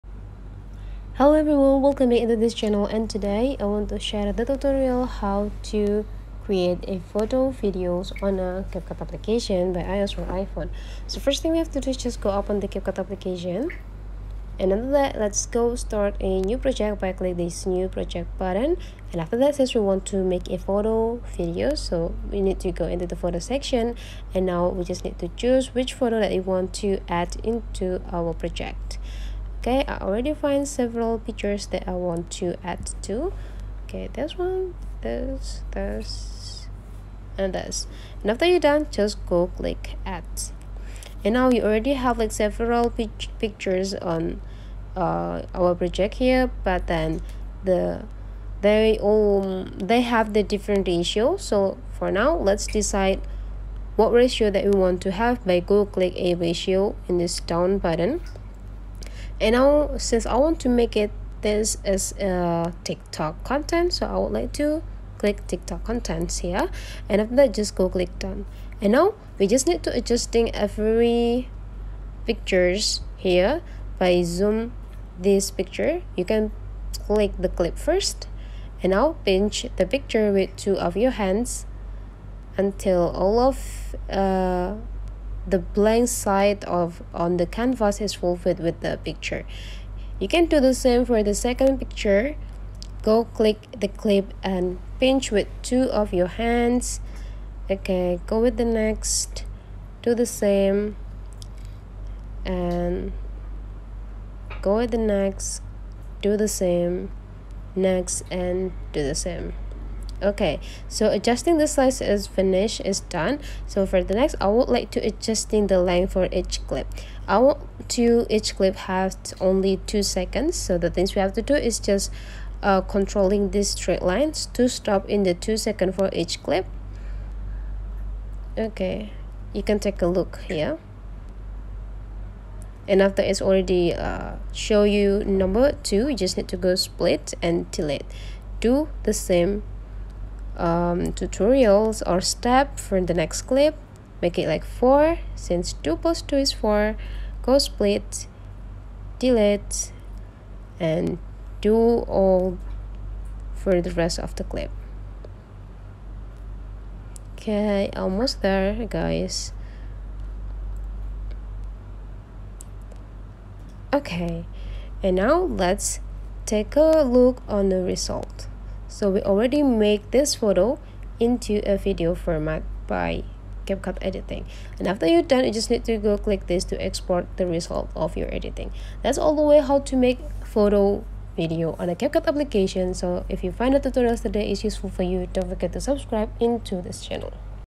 Make a Video from a Bunch of Photos! How to Create Photo Video in Capcut by iOS iPhone?
Make a video from a bunch of your picture! You can create a video by adding several pictures. in the end you will have the video. Here is how to create video from photos.